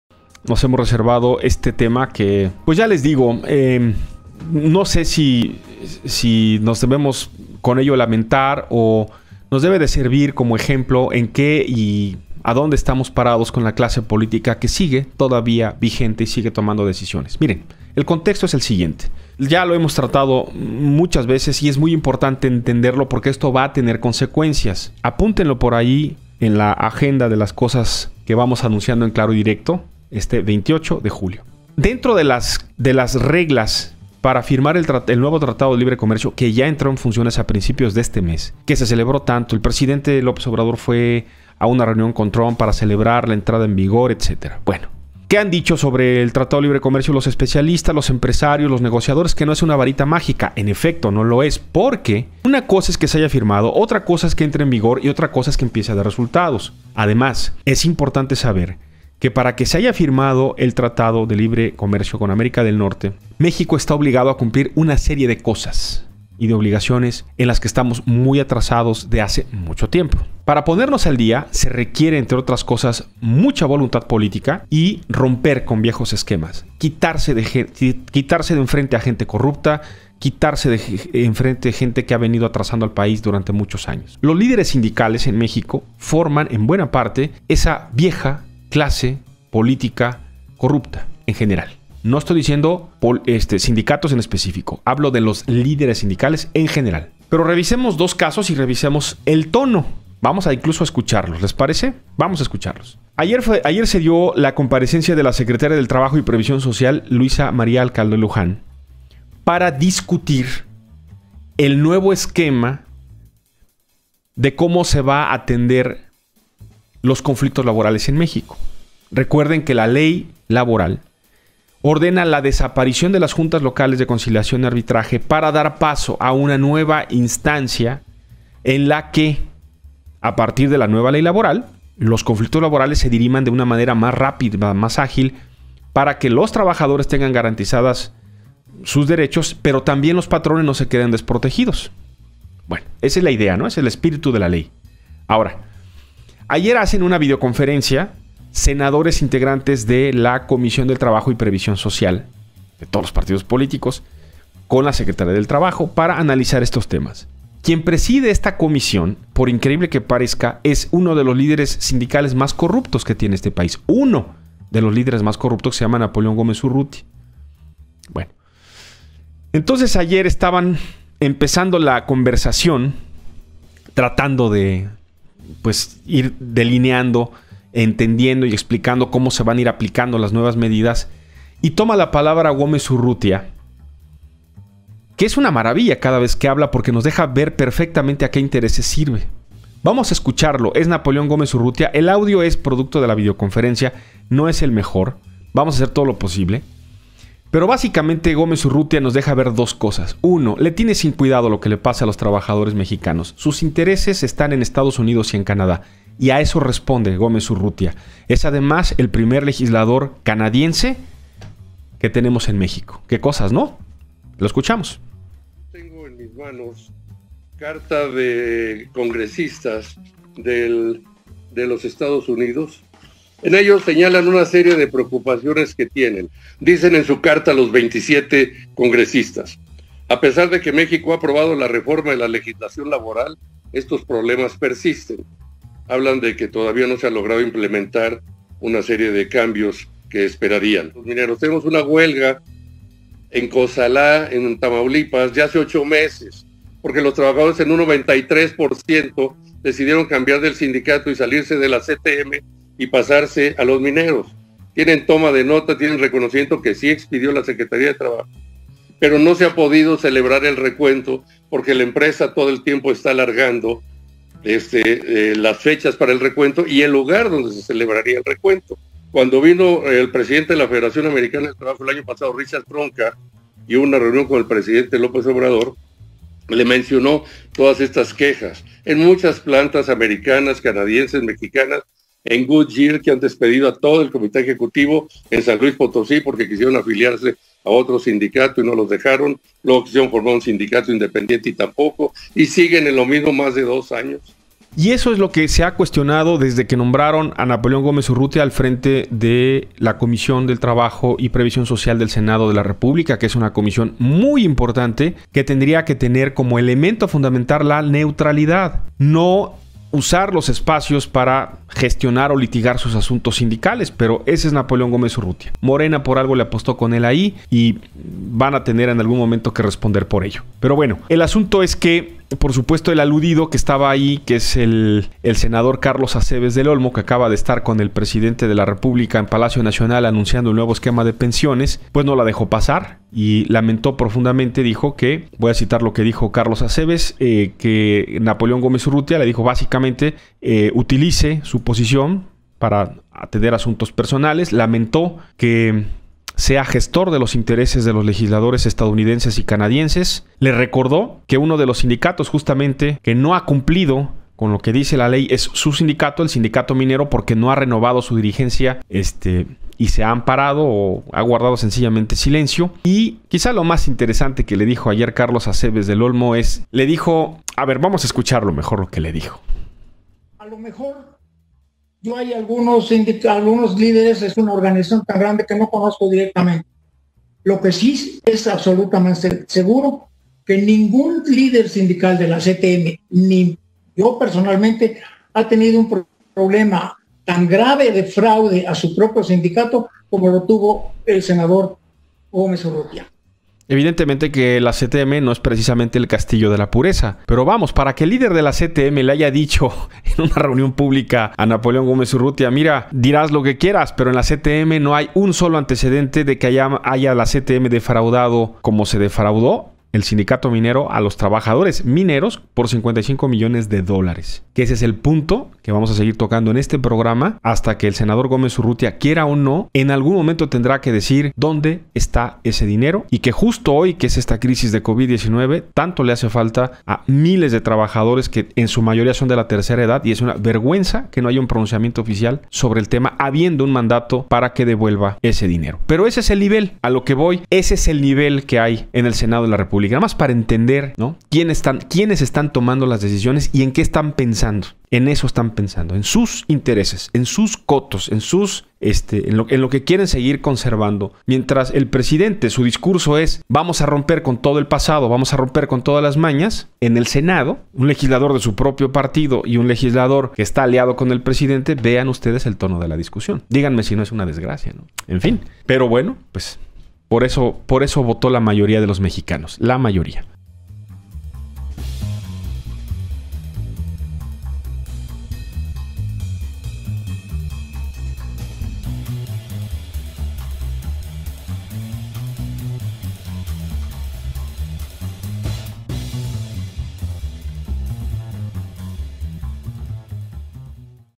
Los senadores Napoleón Gómez Urrutia y Carlos Aceves, arman pleito en videoconferencia
La comparecencia de la secretaria de Trabajo y Previsión Social, Luisa María Alcalde Luján, se convirtió en un ring en el que, el líder del Sindicato Minero, Napoleón Gómez Urrutia, y el líder de la Confederación de Trabajadores de México (CTM), Carlos Aceves del Olmo, se acusaron y dijeron de todo, y evidenciaron sus diferencias por la disputa sin cuartel que tienen en el país. por la titularidad de contratos colectivos en el país.